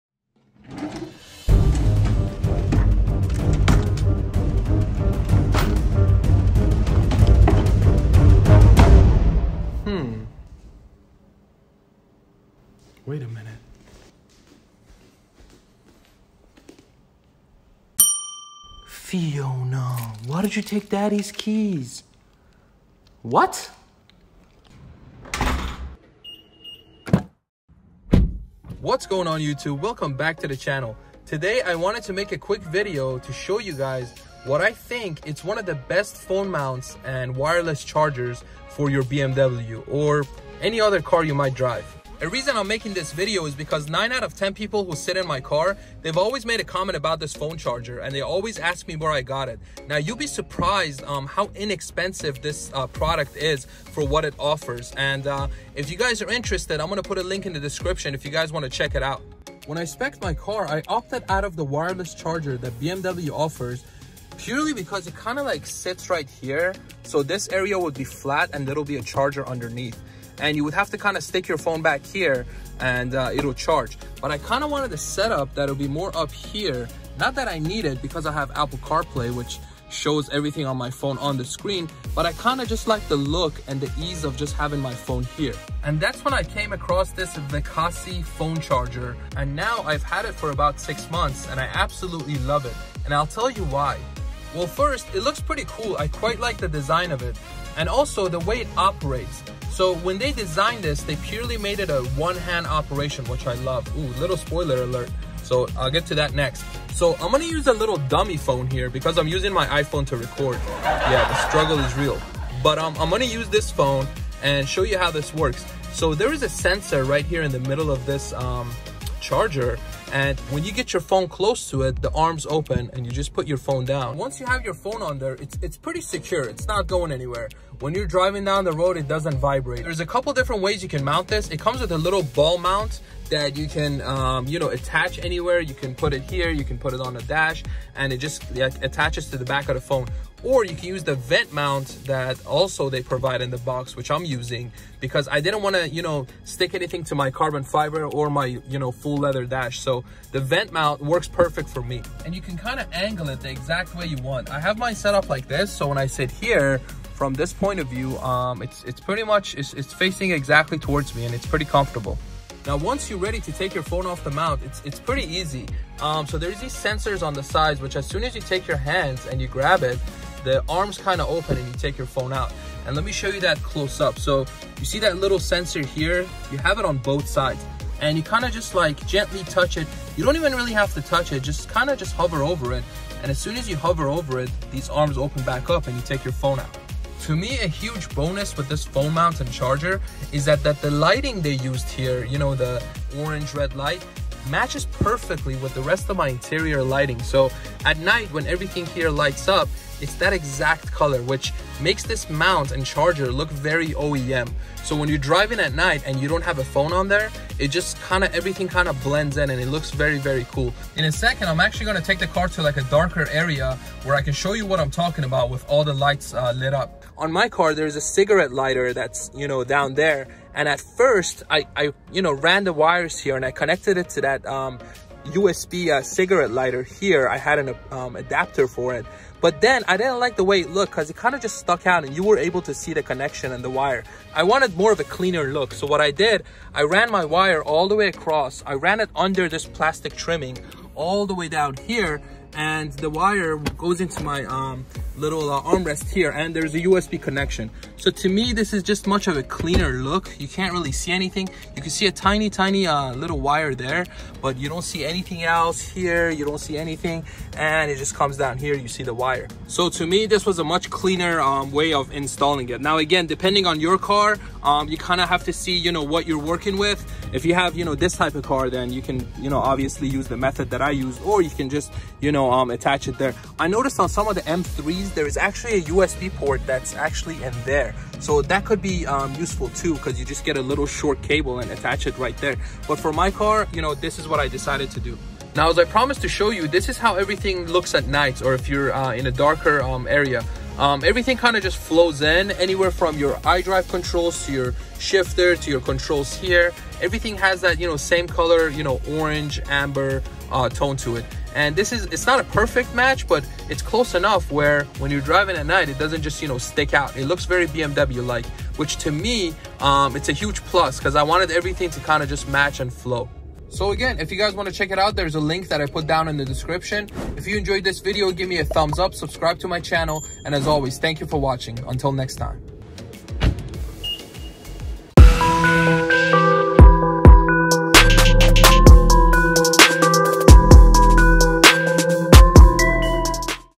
BEST Phone Mount / Wireless Charger for your BMW!
Quick review of the VIKASI 10W Qi fast charging phone mount! I love this thing. Let me know what you think.

Thanks to Tommy L Garage for showing me this mount!

VIKASI Phone Mount

And shout out to little Fiona for turning 3 yesterday and thinking its ok to take my keys and wallet now that she's 21( in dog years).